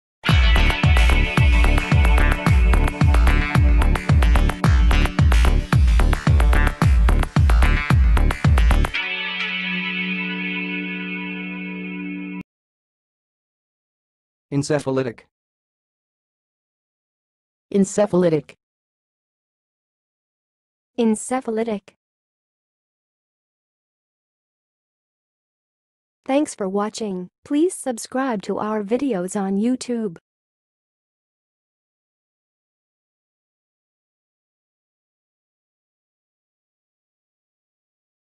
ENCEPHALITIC - HOW TO PRONOUNCE IT?
ENCEPHALITIC - Say Encephalitic like an expert: Pronunciation guide in 2023

Encephalitic is an adjective that describes a condition or disease characterized by inflammation of the brain, known as encephalitis.
Encephalitis can be caused by various factors including infections, such as viral or bacterial infections, autoimmune disorders, or other underlying medical conditions.
The inflammation of the brain can lead to a range of symptoms, including fever, headache, confusion, seizures, and in severe cases, even coma or death.
Treatment for encephalitis typically involves addressing the underlying cause and providing supportive care to manage the symptoms.

Pronunciation:
To pronounce 'encephalitic', you can say [en-sef-uh-LIT-ik].
Alternatively, you can also say [en-sef-uh-LY-tik] or [en-se-fuh-LIT-ik].

Test yourself by speaking the following examples:
1. The child was diagnosed with encephalitic symptoms after exhibiting high fever and confusion
2. It is important to recognize the early signs of encephalitic infections to provide prompt treatment
3. The encephalitic patient experienced seizures as a result of the brain inflammation
4. Researchers are studying new drugs to treat encephalitic disorders
5. The doctor explained the potential complications of encephalitis to the patient's family